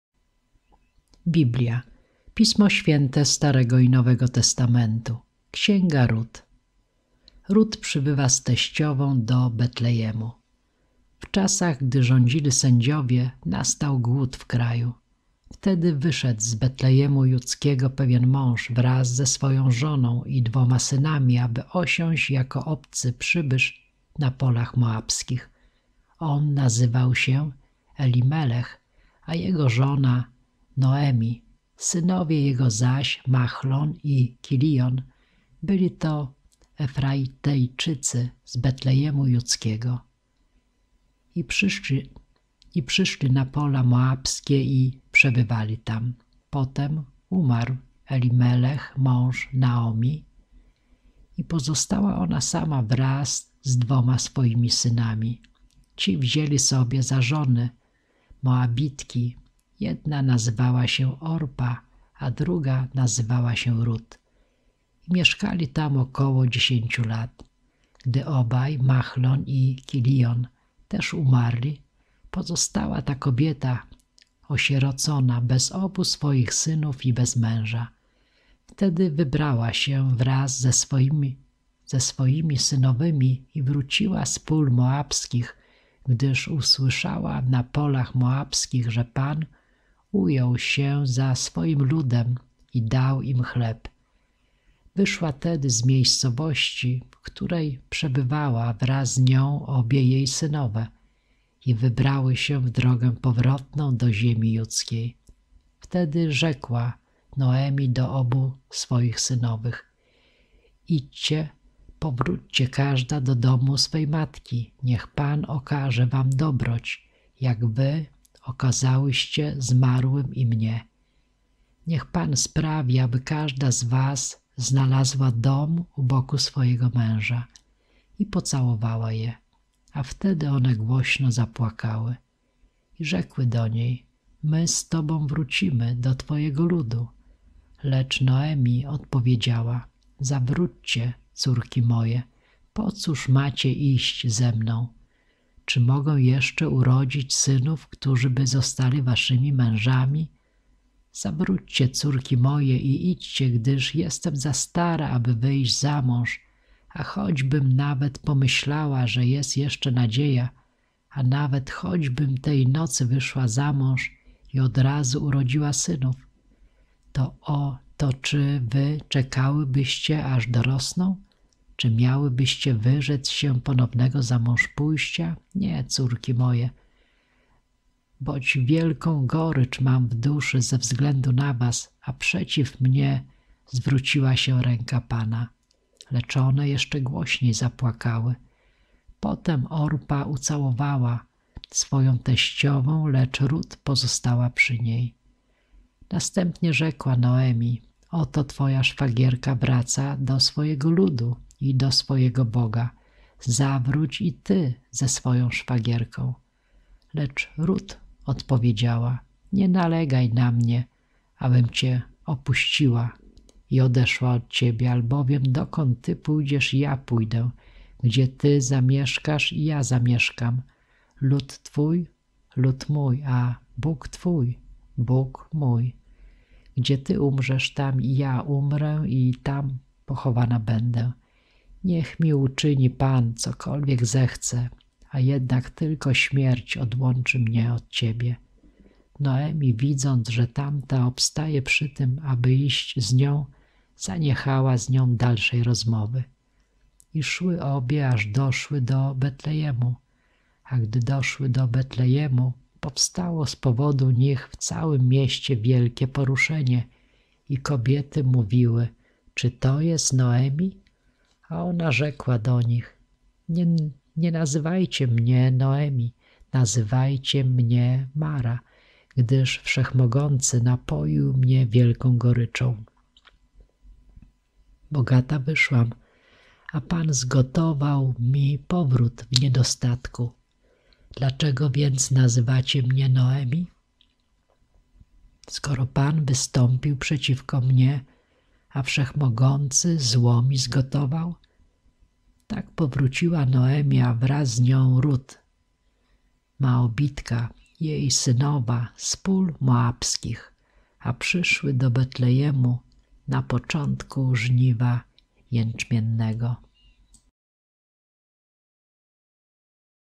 Biblia Stary Testament Księga Ruth -1
Rut przybywa z teściową do Betlejemu